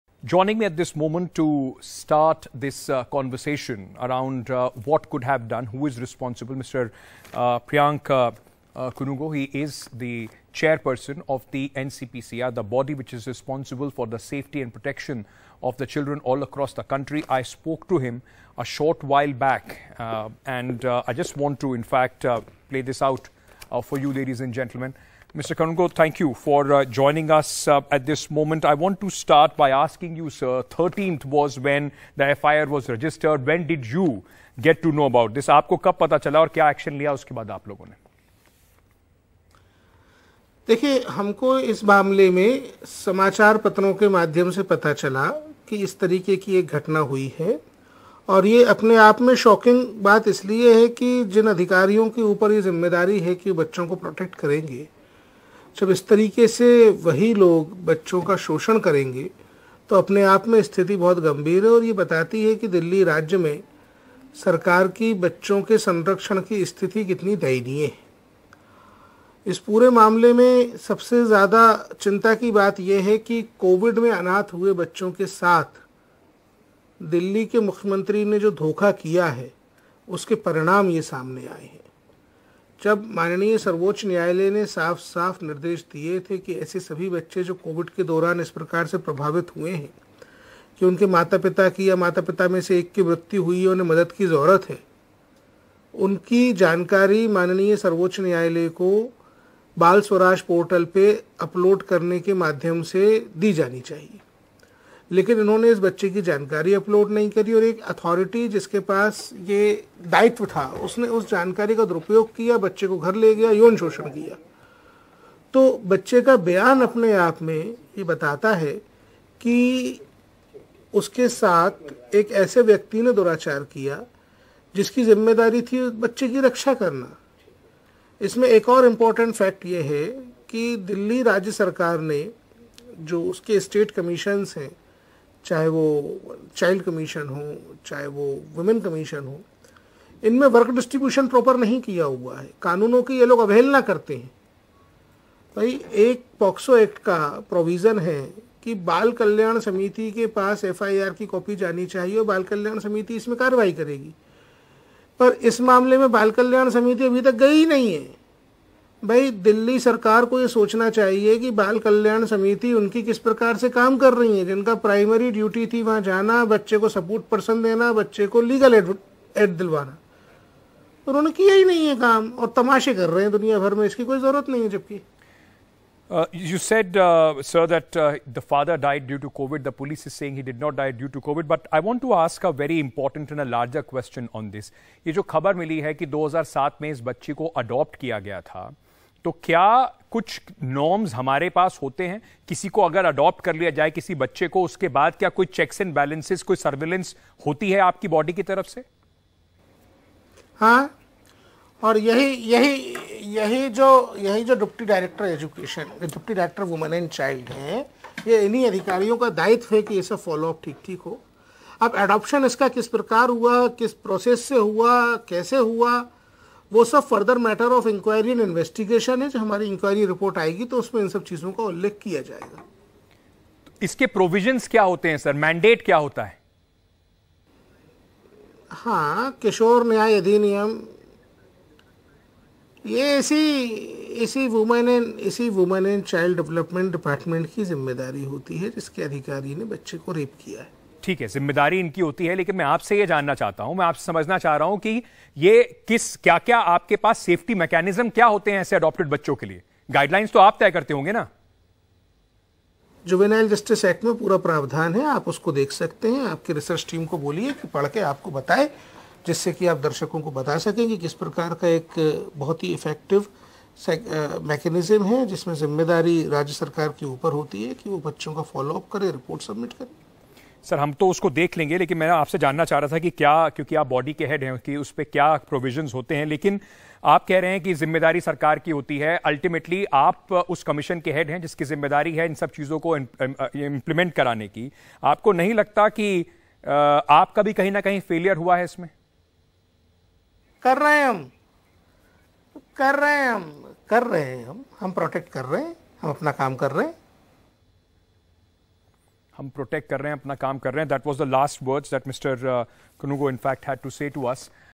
'Security Of Delhi Children In Very Bad State': Child Rights' Body Chief | Breaking Views
Speaking about the horrific rape of a teenager allegedly by a Delhi government official, NCPCR Chairperson Priyank Kanoongo said the incident "shows that the security of children in Delhi is in a very bad state".